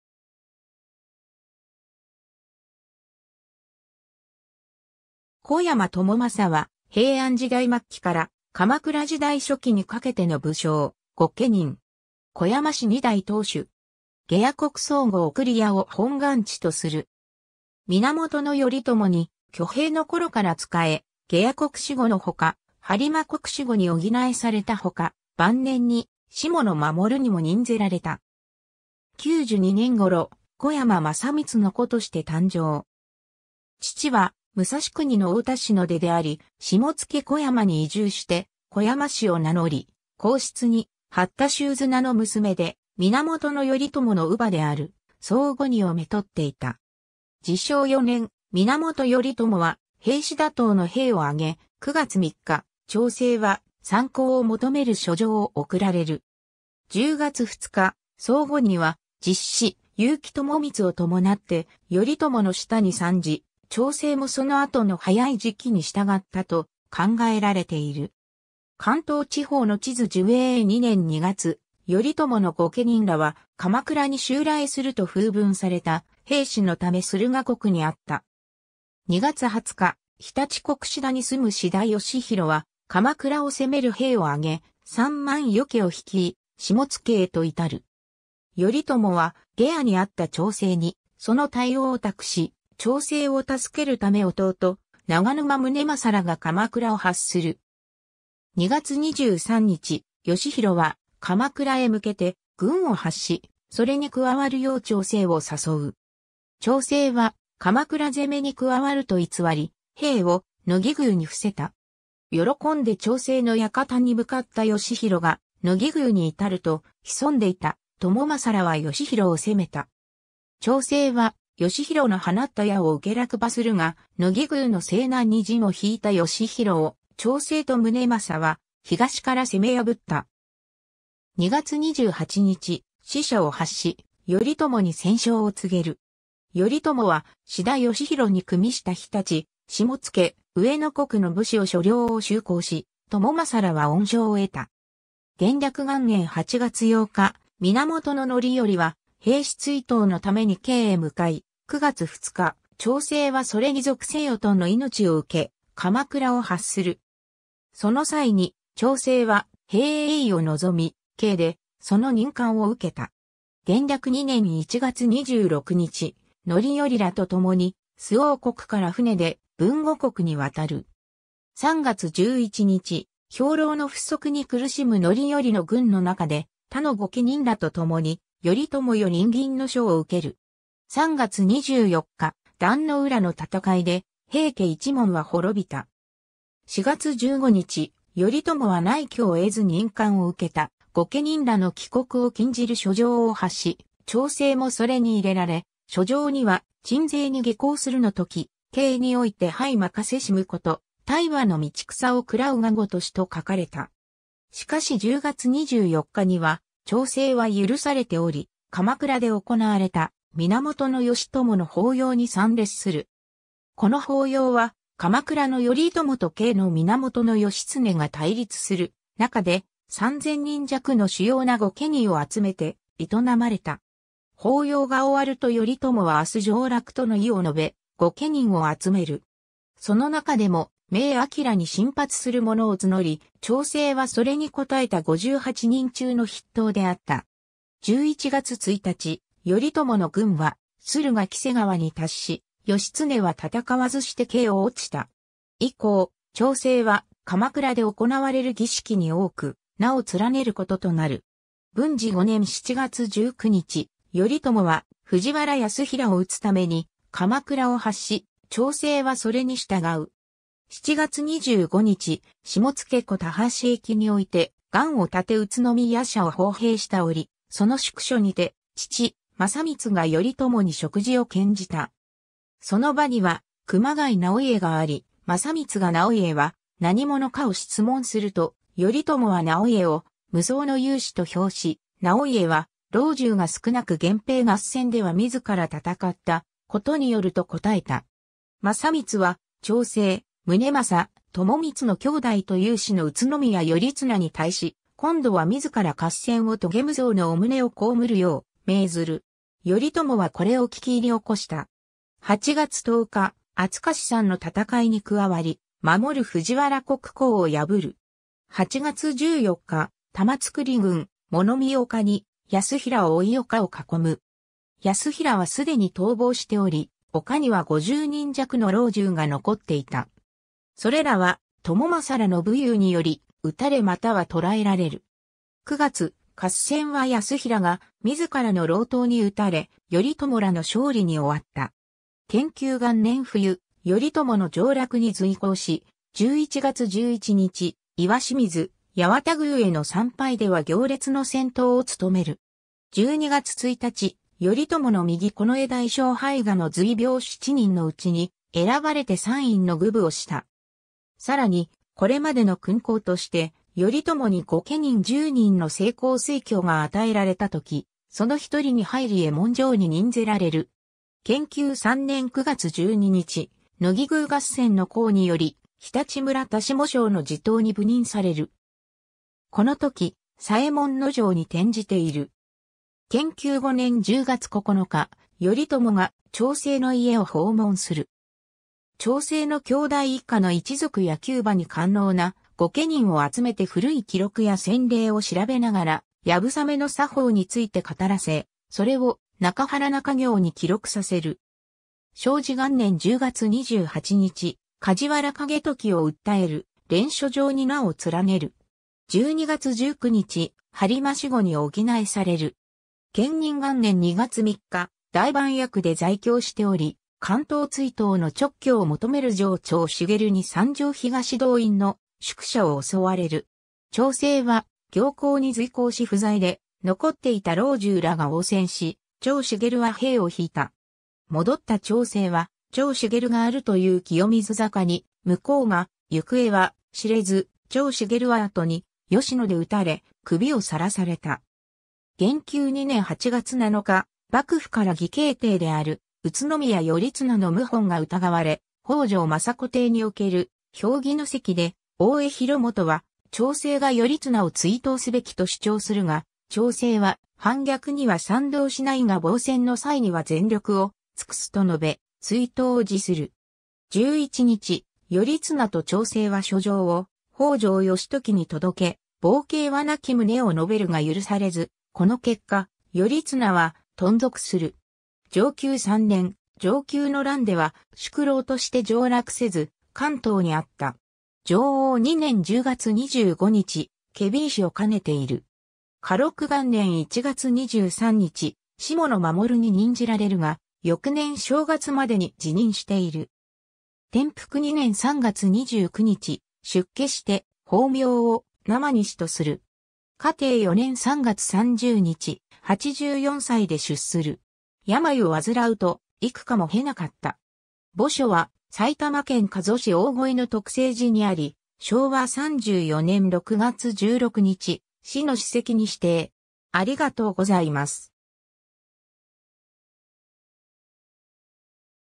小山朝政
小山 朝政（おやま ともまさ）は、平安時代末期から鎌倉時代初期にかけての武将・御家人。
小山氏2代当主。
 下野国寒河御厨（小山庄）を本貫地とする。
源頼朝に挙兵の頃から仕え、下野国守護の他、播磨国守護に補されたほか、晩年に下野守にも任ぜられた。
 久寿2年（1155年）頃、小山政光の子として誕生。
父は武蔵国の太田氏の出であり、下野小山に移住して小山氏を名乗り、後室に八田宗綱の娘で源頼朝の乳母である寒河尼を娶っていた。
 治承4年（1180年）、源頼朝は平氏打倒の兵を挙げ、9月3日、朝政は参向を求める書状を送られる。
10月2日、寒河尼は実子・結城朝光を伴って頼朝の下に参じ、朝政もその後の早い時期に従ったと考えられている。
 関東地方の地図 寿永2年（1183年）2月、頼朝の御家人らは鎌倉に襲来すると風聞された平氏のため駿河国に在った。
2月20日、常陸国志田に住む志田義広は鎌倉を攻める兵を挙げ、三万余騎を率い下野へと到る。
頼朝は下野に在った朝政にその対応を託し、朝政を助けるため弟・長沼宗政らが鎌倉を発する。
2月23日、義広は鎌倉へ向けて軍を発し、それに加わるよう朝政を誘う。
朝政は鎌倉攻めに加わると偽り、兵を野木宮に伏せた。
喜んで朝政の館に向かった義広が野木宮に至ると、潜んでいた朝政らは義広を攻めた。
朝政は義広の放った矢を受け落馬するが、野木宮の西南に陣を引いた義広を、朝政と宗政は東から攻め破った（野木宮合戦）。
 2月28日、使者を発し頼朝に戦勝を告げる。
頼朝は志田義広に与した常陸・下野・上野国の武士を所領を収公し、朝政らは恩賞を得た。
 元暦元年（1184年）8月8日、源範頼は平氏追討の為に京へ向い、9月2日、朝政はそれに属せよとの命を受け鎌倉を発する。
その際に朝政は兵衛尉を望み、京でその任官を受けた。
元暦2年（1185年）1月26日、範頼らと共に周防国から船で豊後国に渡る。
3月11日、兵糧の不足に苦しむ範頼の軍の中で、他の御家人らと共に頼朝より慇懃の書を受ける。
3月24日、壇ノ浦の戦いで平家一門は滅びた。
 4月15日、頼朝は内挙を得ず任官を受けた御家人らの帰国を禁じる書状を発し、朝政もそれに入れられ書状には「鎮西に下向するの時、京に於いて拝任せしむ事、駘馬の道草を喰らうが如し」と書かれた。
しかし10月24日には朝政は許されており、鎌倉で行われた源義朝の法要に参列する。
この法要は鎌倉の頼朝と京の源義経が対立する中で、三千人弱の主要な御家人を集めて営まれた。
法要が終わると頼朝は明日上洛との意を述べ御家人を集める。
その中でも明暁に進発する者を募り、朝政はそれに応えた58人中の筆頭であった。
11月1日、頼朝の軍は駿河黄瀬川に達し、義経は戦わずして京を落ちた。
以降、朝政は鎌倉で行われる儀式に多く名を連ねる事となる。
 文治5年（1189年）7月19日、頼朝は藤原泰衡を討つ為に鎌倉を発し、朝政はそれに従う。
7月25日、下野古多橋駅（現・宇都宮市）において願を立て宇都宮社を奉幣した折、その宿所にて父・政光が頼朝に食事を献じた。
その場には熊谷直家が在り、政光が直家は何者かを質問すると、頼朝は直家を無双の勇士と評し、直家は郎従が少く源平合戦では自ら戦った事によると応えた。
政光は朝政・宗政・朝光の兄弟と猶子の宇都宮頼綱に対し、今度は自ら合戦を遂げ無双の御旨を蒙るよう命ずる。
頼朝はこれを聞き入興（じゅきょう）した。
 8月10日、阿津賀志山の戦いに加わり、守る藤原国衡を破る。
8月14日、玉造郡物見岡に泰衡を追い岡を囲む。
泰衡は既に逃亡しており、岡には50人弱の郎従が残っていた。
それらは朝政らの武勇により、討たれまたは捕らえられる。
9月、合戦は泰衡が自らの郎党に討たれ、頼朝らの勝利に終わった。
 建久元年（1190年）冬、頼朝の上洛に随行し、11月11日、石清水八幡宮への参拝では行列の先頭を務める。
12月1日、頼朝の右近衛大将拝賀の随兵7人の内に選ばれて参院の供奉をした。
さらに、これまでの勲功として頼朝に御家人10人の成功推挙が与えられた時、その1人に入り右衛門尉に任ぜられる。
 建久3年（1192年）9月12日、野木宮合戦の功により常陸村田下庄の地頭に補任される。
この時、左衛門尉に転じている。
建久5年（1194年）10月9日、頼朝が朝政の家を訪問する。
朝政の兄弟以下の一族や弓馬に堪能な御家人を集めて古い記録や先例を調べながら流鏑馬の作法...